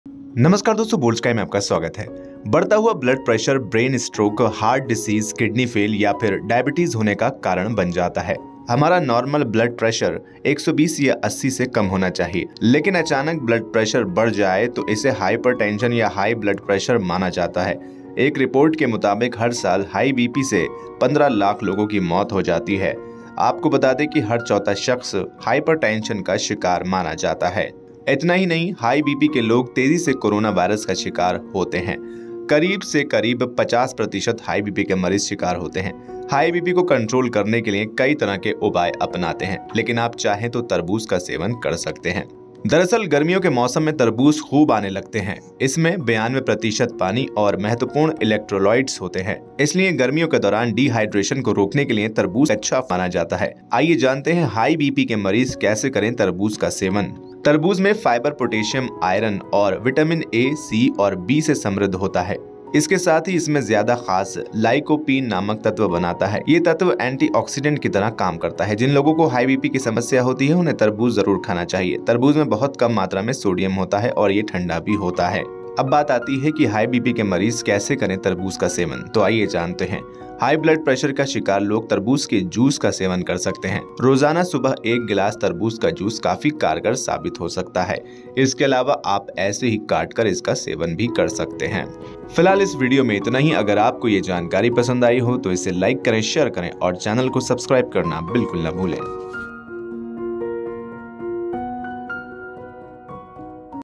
Watermelon से High Blood Pressure कैसे Control होता है | High BP में Watermelon खाने से क्या होता है
तरबूज में 92 प्रतिशत पानी और महत्वपूर्ण इलेक्ट्रोलाइट्स होते हैं, इसलिए गर्मियों के दौरान डिहाइड्रेशन को रोकने के लिए तरबूज एक अच्छा फूड माना जाता है। जानिए हाी बीपी के मरीज कैसे करें तरबूज का सेवन।

Watermelon contains 92 percent water and important electrolytes, so watermelon is a great food to prevent dehydration during summers. Know how high BP patients should consume watermelon.